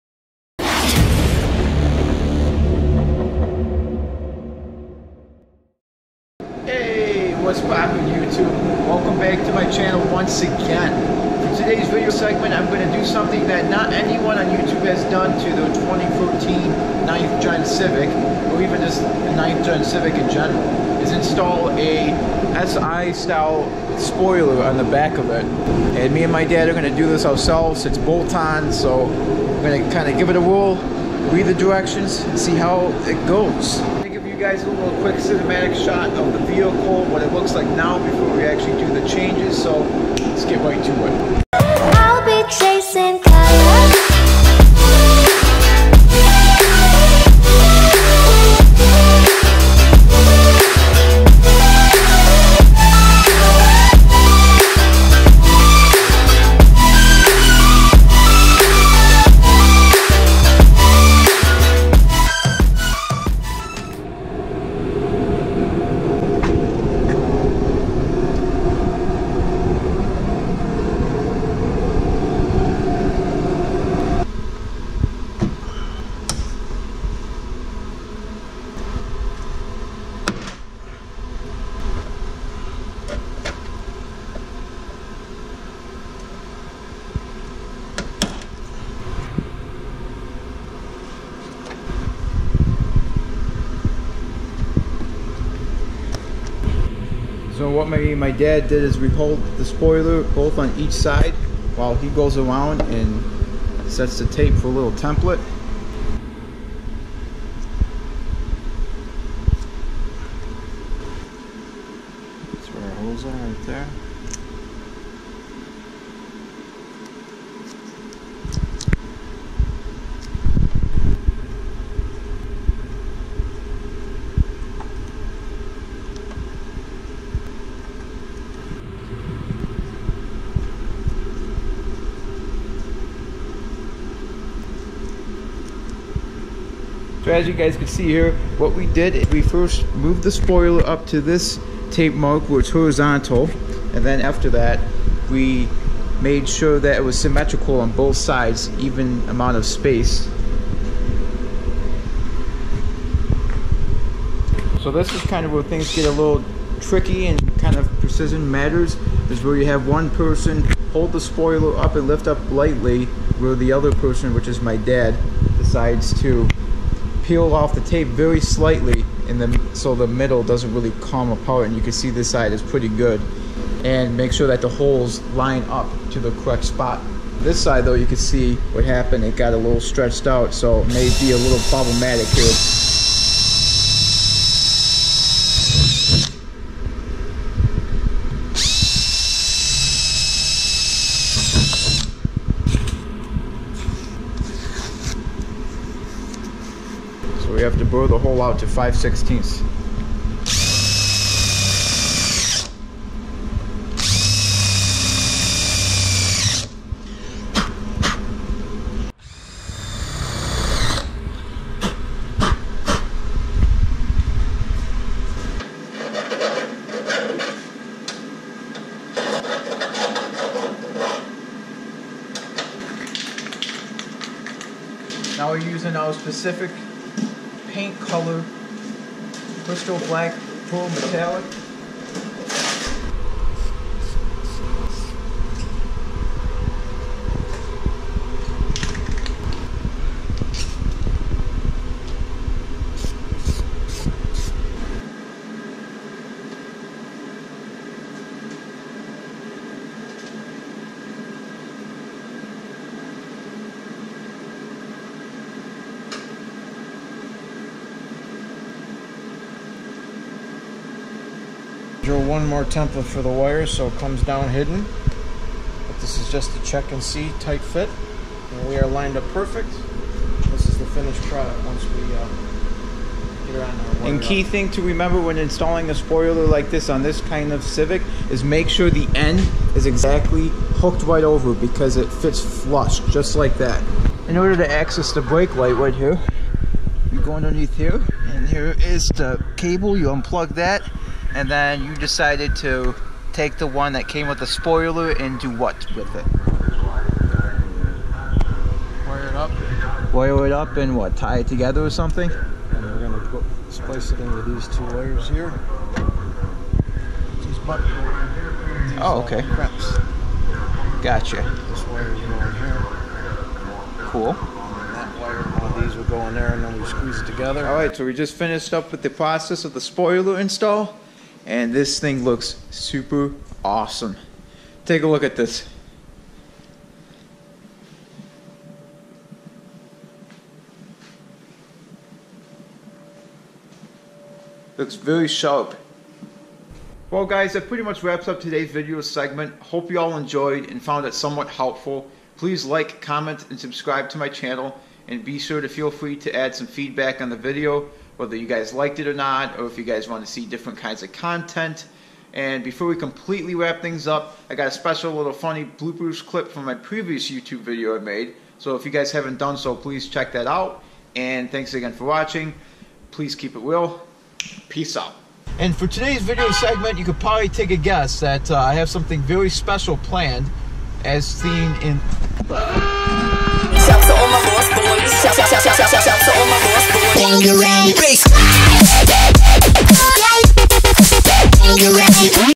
Installing Si Spoiler on 2014 Honda Civic EX
Actual install starts at 1:04!! Cool little intro is at beginning of the video before this. Here is a link to where I got the product:

The actual spoiler came in excellent packaging and overall condition as one consumer should expect in a delivery. Only unfortunate thing about this website is the shipping cost 20 dollars alone. However, it came right on time. As far as I know, the Si style spoilers can fit non si modeled Civics but the spoiler has to fit with the body style you have. I have stated 2 times in the video that the spoiler needs to be hanging over the rear trunk. This is because that is how the spoiler is meant to sit. Doing any other way is possible but then you would have to buy new longer screws and possibly drill through components inside the trunk which may lead into major problems. Spoiler wont sit completely sit flat if done the other way than what was portrayed in the video. Next video coming in the next month!